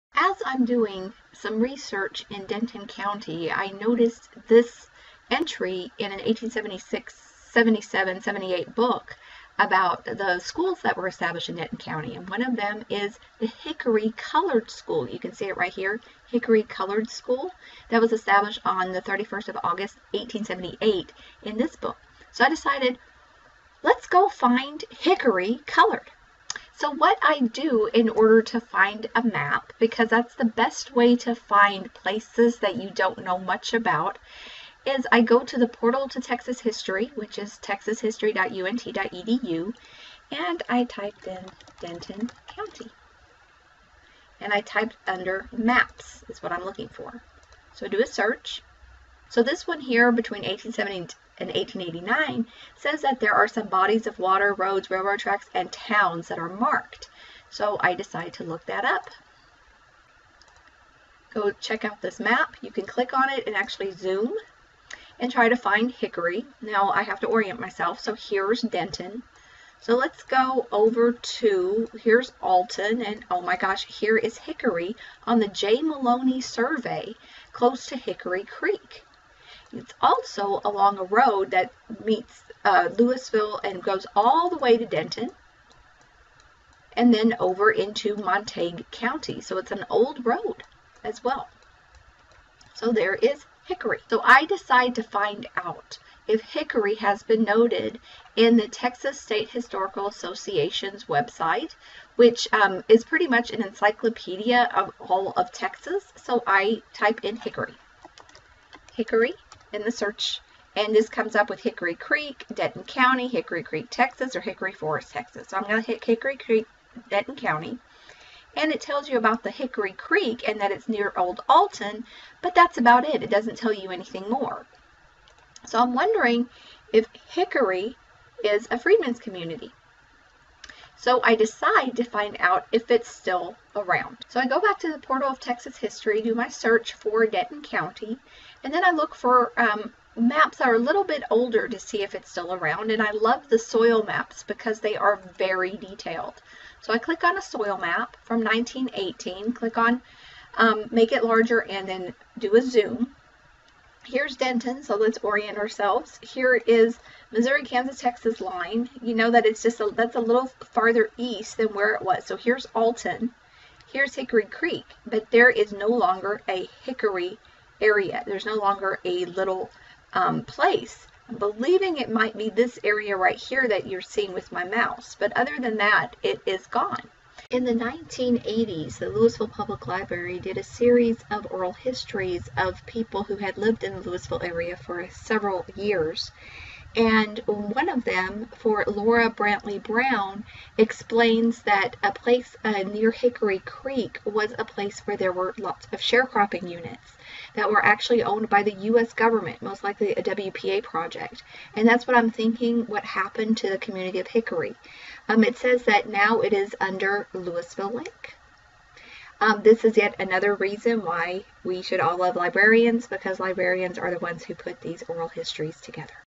Map Discoveries = Denton County Freedmen Community
The creation of the "Hickory Colored School" in Denton County led me to wonder, where was it located? Using maps, I discovered where the little community used to be.

Sources: Portal to Texas History, Texas State Historical Association, Denton County Office of History and Culture, Lewisville Public Library